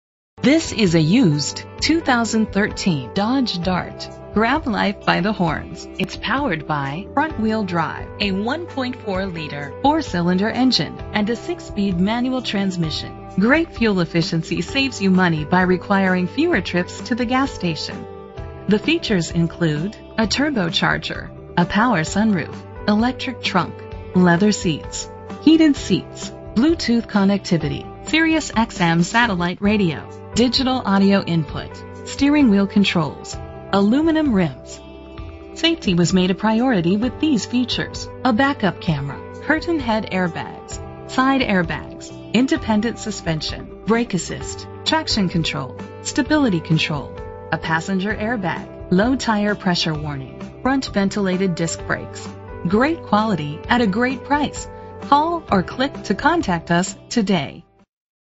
2013 Dodge Dart - Mattoon IL
Pilson Auto
Year: 2013
Make: Dodge
Model: Dart
Trim: Limited/GT
Engine: 1.4 liter inline 4 cylinder 16 valve
Transmission: 6-Speed Manual
Color: Tungsten Clearcoat Metallic
Mileage: 36026
Address:
2212 Lake Land Blvd.